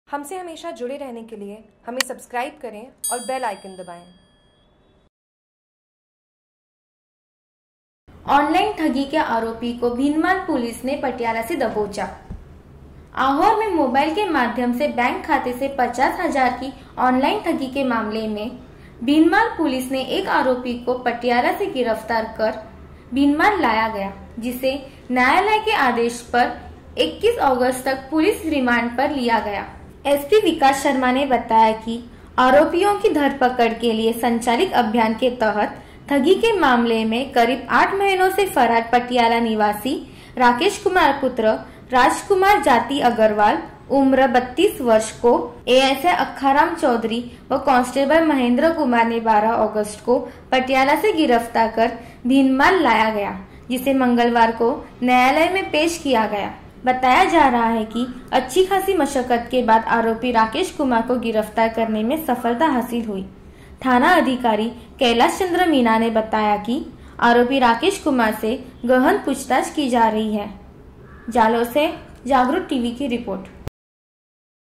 JAGRUK TV:जालोर:ऑनलाईन ठगी के मामले में आरोपी गिरफ्तार
जालोर
आहोर में मोबाईल के माध्यम से बैंक खाते से 50 हजार की ऑनलाईन ठगी के मामले में भीनमाल पुलिस ने एक आरोपी को पटियाला (पंजाब) से गिरफ्तार कर भीनमाल लाया गया। जिसे न्यायालय के आदेश पर 21 अगस्त तक पुलिस रिमांड पर लिया गया। एसपी विकास शर्मा ने बताया कि अपराधियों की धरपकड के लिए संचालित अभियान के तहत ठगी के मामले में करीब आठ माह से फरार पटियाला (पंजाब) निवासी राकेश कुमार पुत्र राजकुमार जाति अग्रवाल (गर्ग) उम्र 32 वर्ष को एएसआई अखाराम चौधरी व काँस्टेबल महेन्द्रकुमार ने 12 अगस्त को पटियाला से दस्तयाब कर भीनमाल लाया गया। जिसे मंगलवार को न्यायालय में पेश किया गया। जहा से 21 अगस्त तक पुलिस रिमांड पर भेजने के आदेश दिए। ज्ञात रहे की आहोर निवासी मांगीलाल पुत्र ताराचंद जैन ने 20 अक्टूम्बर 2017 को पुलिस थाना आहोर में मामला दर्ज करवाया था कि उसके मोबाईल नंबर पर अज्ञात व्यक्ति का फोन आया। जिसने स्वय को बैंक मैनेजर बताते हुए फोन कर एटीएम से संबंधित जानकारी प्राप्त कर उसके खाते से ऑनलाईन 50 हजार रुपये निकाल दिए। पुलिस विभाग द्वारा साईबर क्राईम की रोकथाम के लिए नियुक्त नोडल पुलिस थाना भीनमाल को उक्त मामला जाँच के लिए भेजा गया । जिस पर थाना अधिकारी कैलाशचन्द्र मीणा द्वारा अनुसंधान के दौरान साईबर सैल की तकनीकी सहायता के माध्यम से अज्ञात व्यक्ति को नामजद कर उसकी गिरफ्तारी के लिए एएसआई अखाराम चौधरी व काँस्टेबल महेन्द्रकुमार के नेतृत्व में पुलिस दल को पटीयाला (पंजाब) भेजा गया। जहा खासी मशकत के बाद आरोपी राकेशकुमार को दस्तयाब करने में सफलता हासिल की। थाना अधिकारी कैलाशचन्द्र मीणा ने बताया कि आरोपी राकेश कुमार से गहन पूछताछ की जारी हैं।